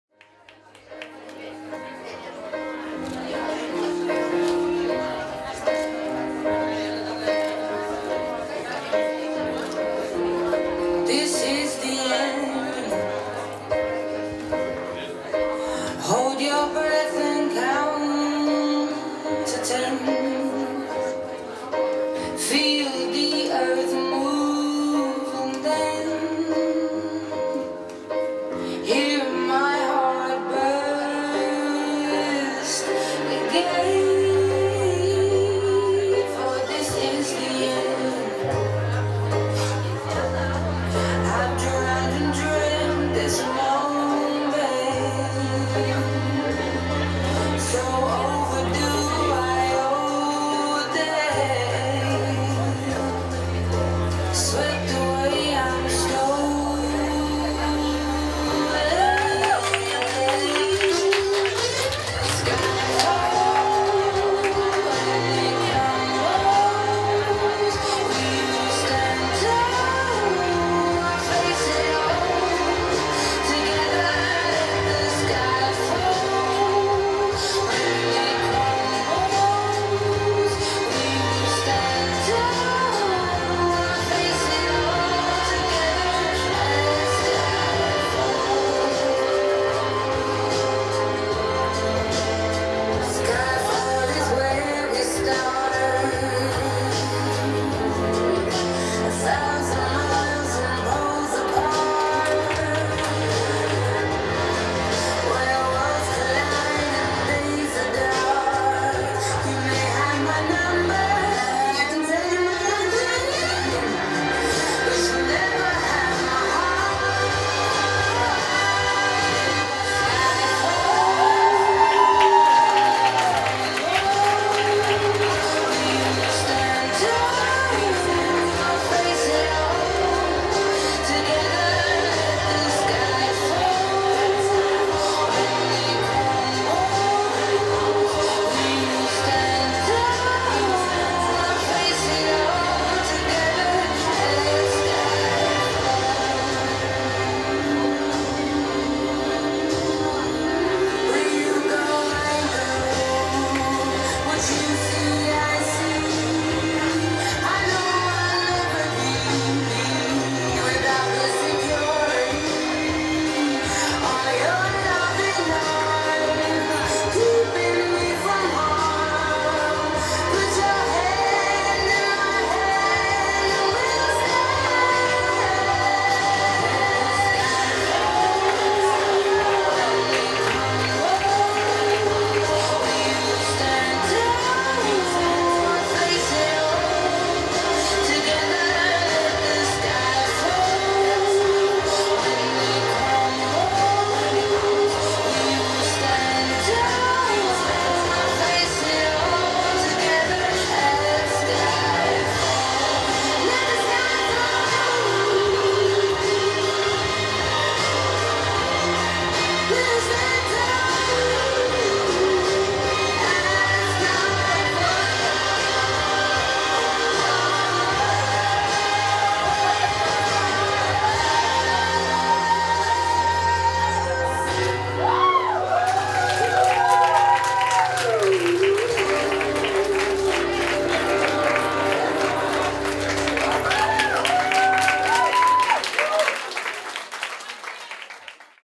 CatWalk Dance Fest, Чижова Анастасия.
7 апреля 2013г., первый CatWalk Dance Fest.
Чижова Анастасия, 3е место в номинации AERIAL SILK среди профессионалов.

КэтВолк - танец в воздухе и на земле!

POLE SPORT Professional - генеральный партнер и техническое обеспечение фестиваля.
- пилоны для танца, кольца, полотна.